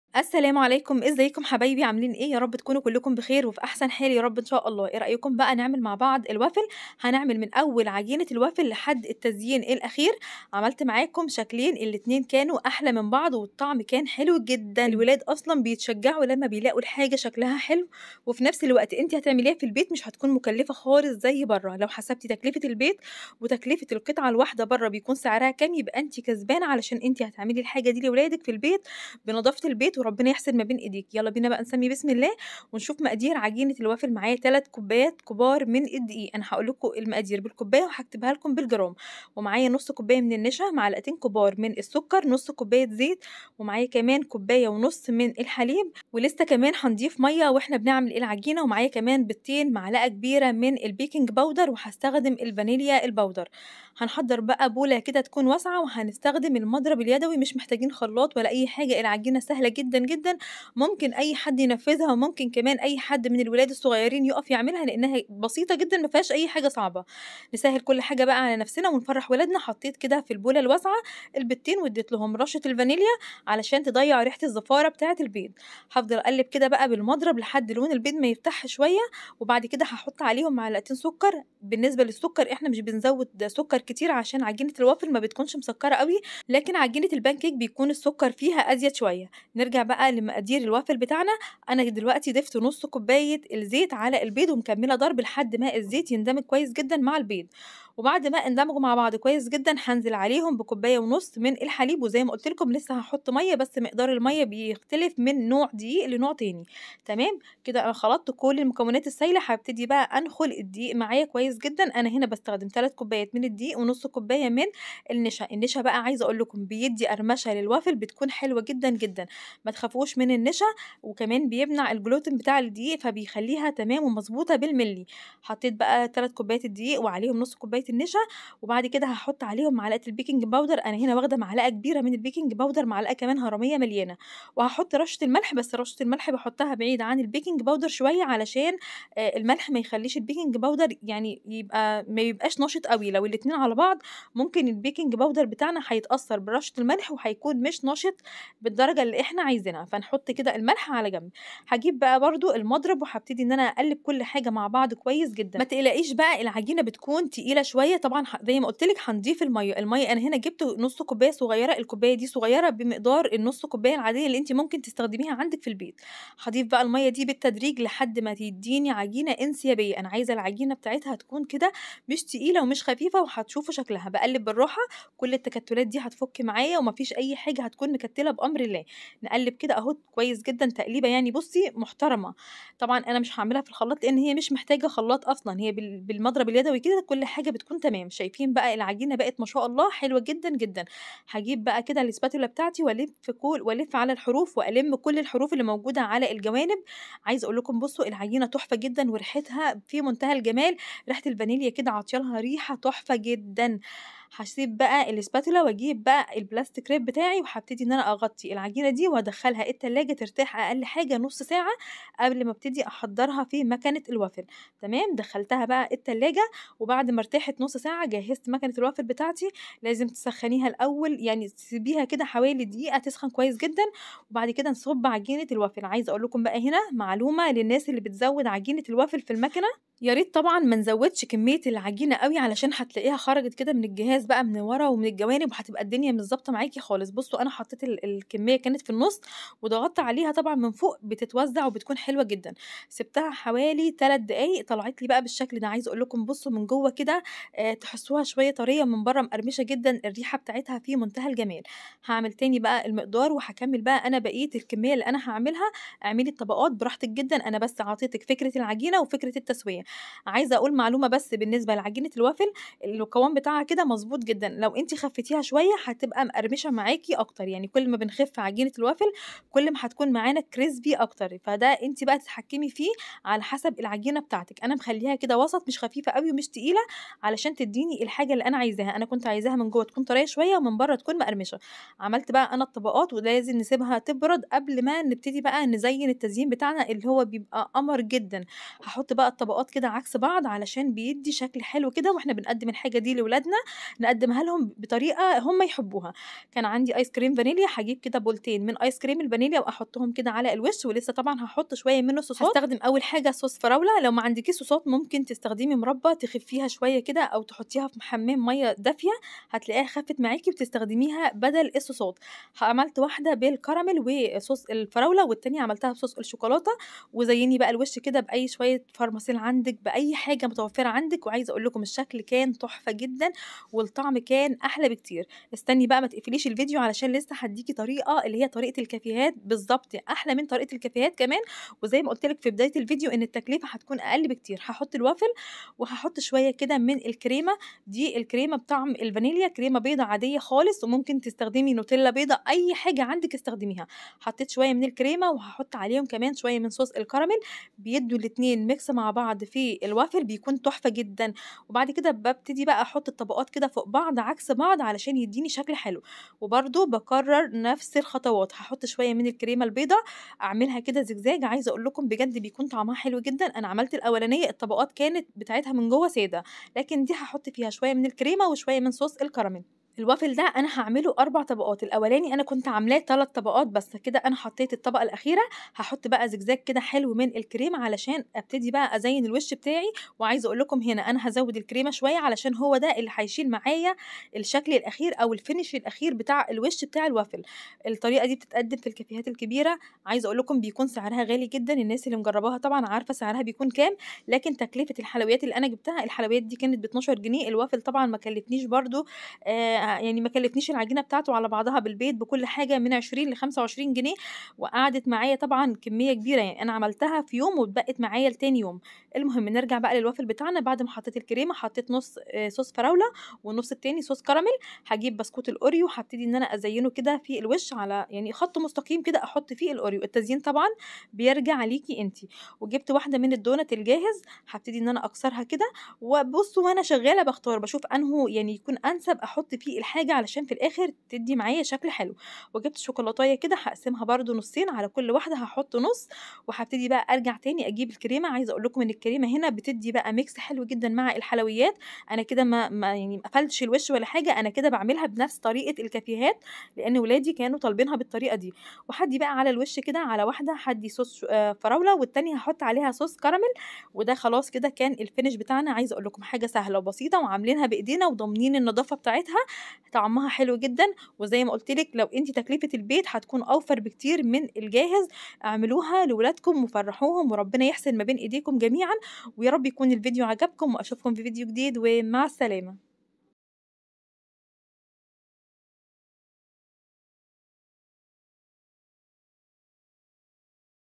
الوافل بأبسط طريقه🧇هش ومقرمش وهينجح معاكي من اول مره
لو ولادك بيحبو الوافل اوعي تشتريه جاهز في البيت حضريه وبأبسط المكونات هنعمله وهينجح معاكي من اول مره